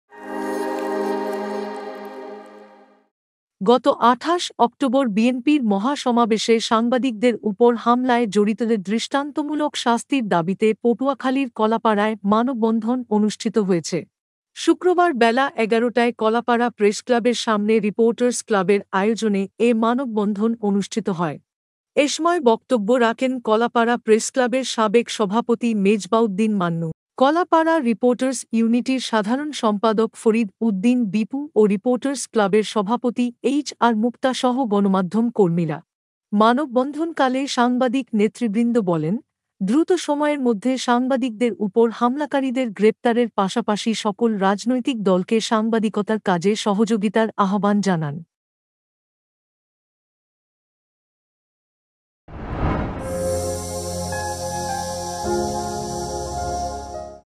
সাংবাদিকদের উপর হামলা; জড়িতদের শাস্তির দাবিতে কলাপাড়ায় মানববন্ধন | Channel S News | Channel S 2023
গত আঠাশ অক্টোবর বিএনপির মহাসমাবেশে ঢাকায় সাংবাদিকদের উপর হামলায় জড়িতদের দৃষ্টান্তমূলক শাস্তির দাবিতে পটুয়াখালীর কলাপাড়ায় মানববন্ধন অনুষ্ঠিত হয়েছে.

1: News :
2: Entertainment News :
3: Sports News :